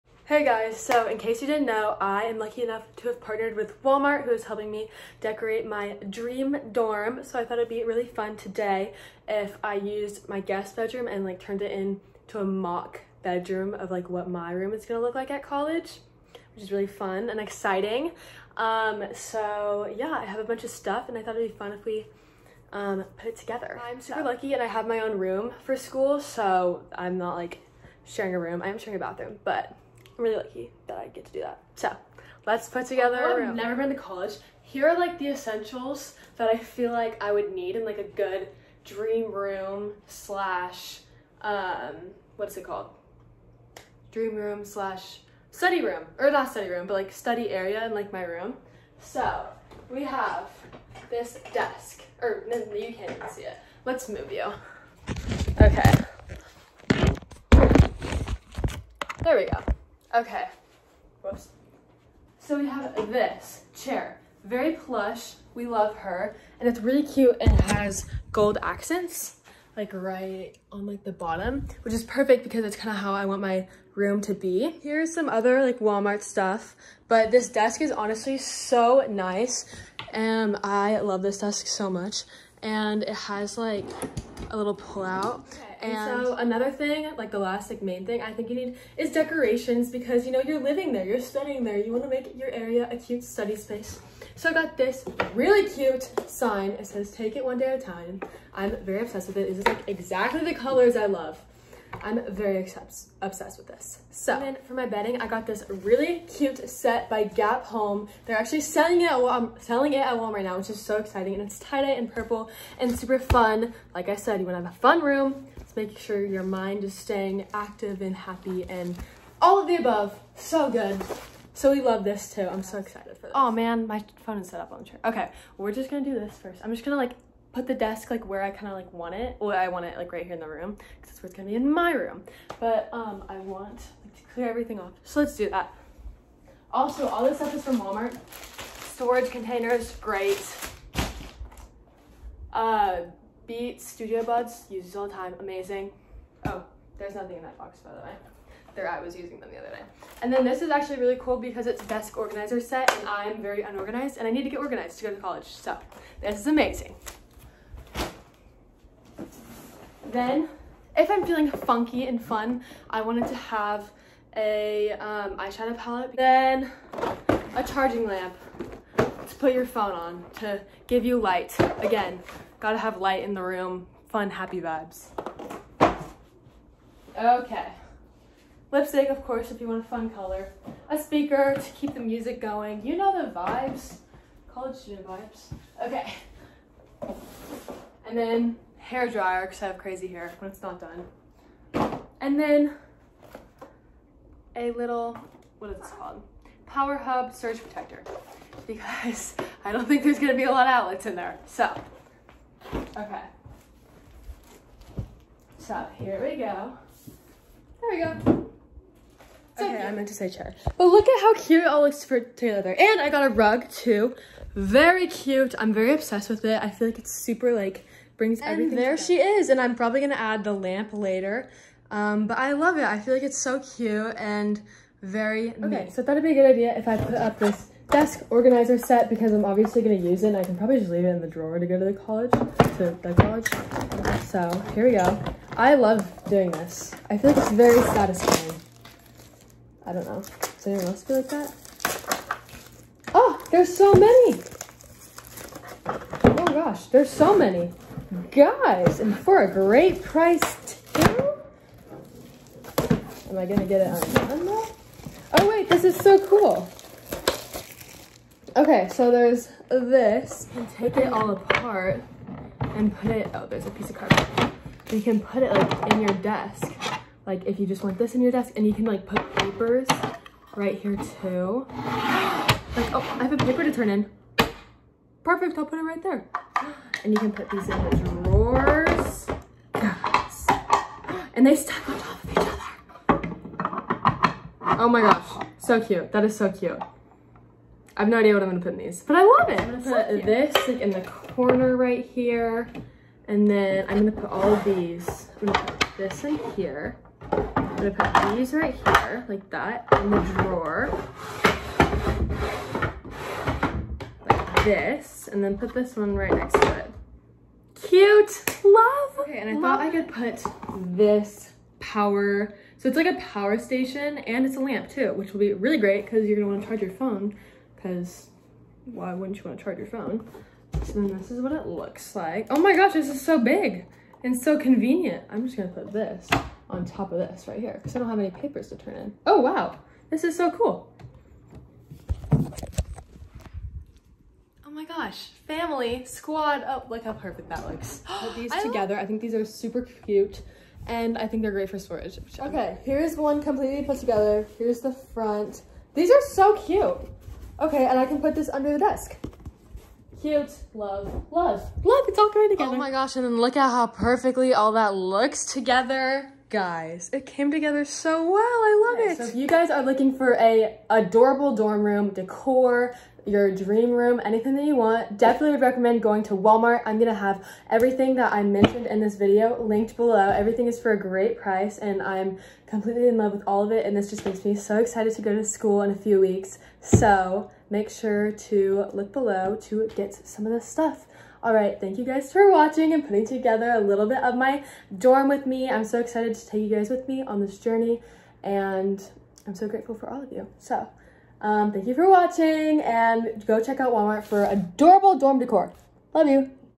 Walmart Dorm Haul!
Thanks to @walmart I have been able to start designing my dream dorm before I actually move in! I feel super prepared and even more excited for the semester with these dorm must-haves. Below are the links to all the products mentioned in this video.